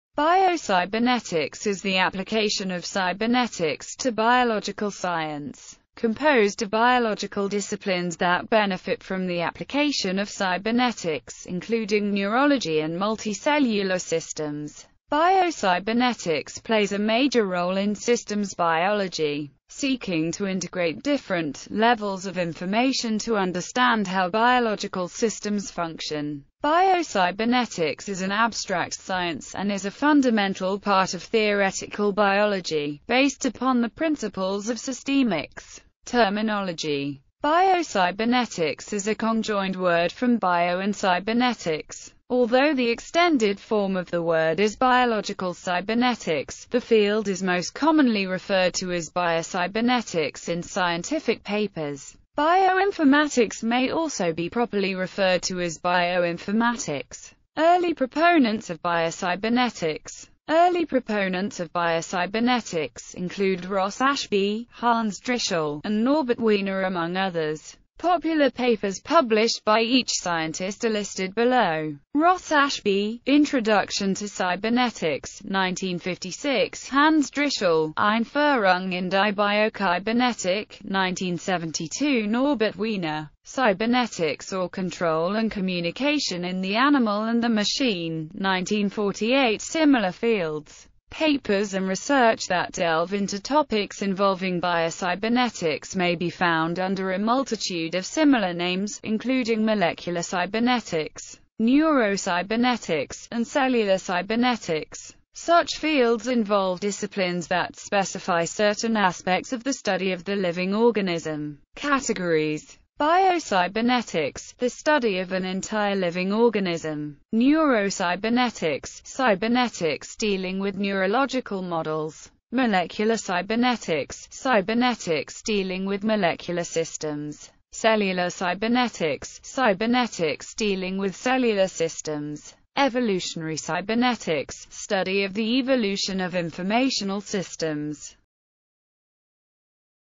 Biocybernetics
Biocybernetics is the application of cybernetics to biological science, composed of biological disciplines that benefit from the application of cybernetics including neurology and multicellular systems.Biocybernetics plays a major role in systems biology, seeking to integrate different levels of information to understand how biological systems function.